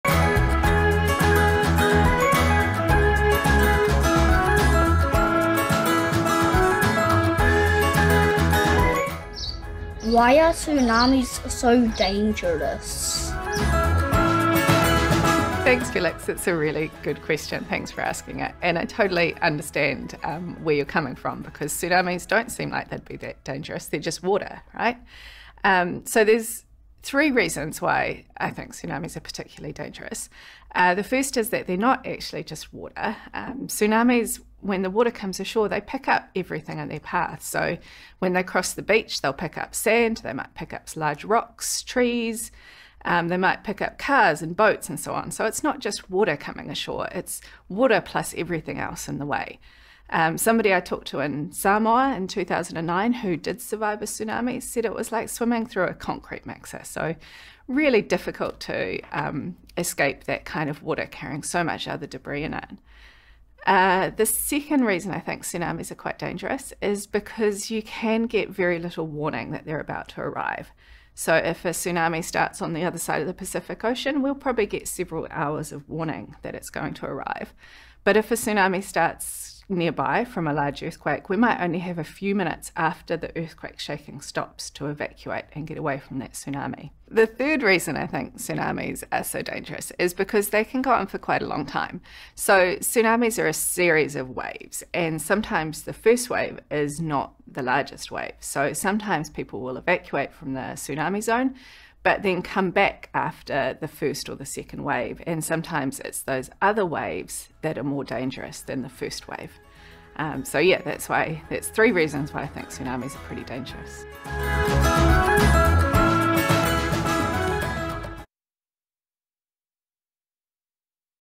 Kids Ask an Expert - Why are tsunamis so dangerous?
Felix, 5, asks our expert Dr Kate Clark why are tsunamis so dangerous.